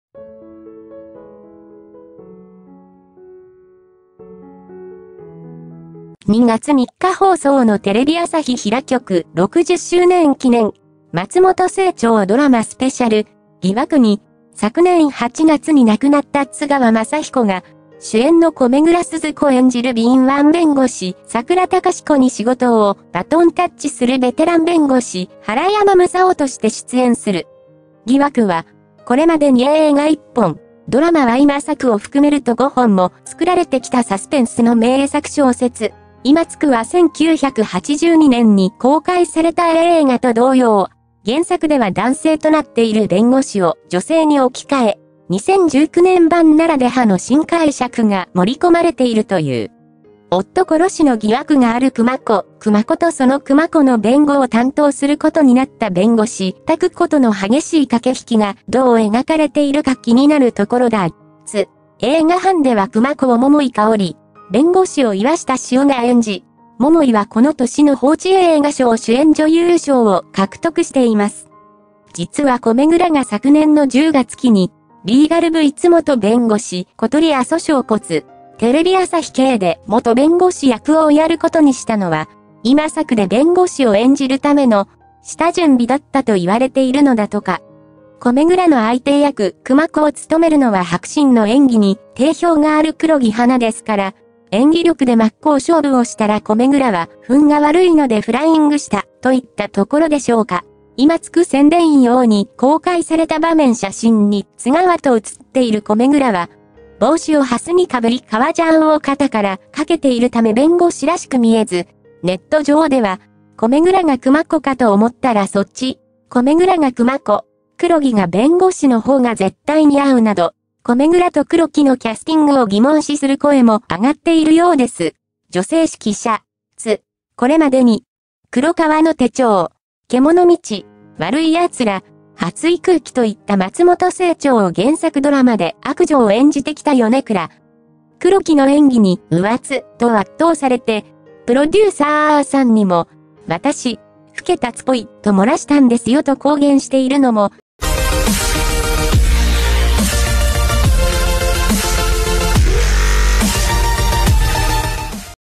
米倉涼子、ドラマ「疑惑」で故・津川雅彦との共演より驚かれた「役どころ」！ - ニュース 速報
米倉涼子、ドラマ「疑惑」で故・津川雅彦との共演より驚かれた「役どころ」！ - ニュース 速報   　2月3日放送のテレビ朝日開局60周年記念「松本清張ドラマスペシャル『疑惑』」に、昨年8月に亡くなった津川雅彦が、主演の米倉涼子演じる敏腕弁護士・佐倉卓子に仕事をバトンタッチするベテラン弁護士・原山正雄として出演する。  「疑惑」は、これまでに映画1本、ドラマは今作を含めると5本も作られてきたサスペンスの名作小説。今作は1982年に公開された映画と同様、原作では男性となっている弁護士を女性に置き換え、2019年版ならではの新解釈が盛り込まれているという。夫殺しの疑惑がある球磨子（くまこ）とその球磨子の弁護を担当することになった弁護士・卓子との激しい駆け引きがどう描かれているか気になるところだ。  「映画版では球磨子を桃井かおり、弁護士を岩下志麻が演じ、桃井はこの年の報知映画賞主演女優賞を獲得しています。実は米倉が昨年の10月期に『リーガルV～元弁護士・小鳥遊翔子～』（テレビ朝日系）で元弁護士役をやることにしたのは、今作で弁護士を演じるための“下準備”だったと言われているのだとか。米倉の相手役・球磨子を務めるのは迫真の演技に定評がある黒木華ですから、演技力で真っ向勝負をしたら米倉は分が悪いのでフライングした、といったところでしょうか。今作宣伝用に公開された場面写真に津川と写っている米倉は、帽子を斜にかぶり革ジャンを肩からかけているため弁護士らしく見えず、ネット上では『米倉が球磨子かと思ったらそっち？』『米倉が球磨子、黒木が弁護士のほうが絶対似合う』など、米倉と黒木のキャスティングを疑問視する声もあがっているようです」（女性誌記者）  　これまでに「黒革の手帳」「けものみち」「わるいやつら」「熱い空気」といった松本清張原作ドラマで悪女を演じてきた米倉。黒木の演技に「『うわっ！』と圧倒されて、プロデューサーさんにも『私、負けたっぽい‥‥』と漏らしたんですよ」と公言しているのも、転ばぬ先の杖か。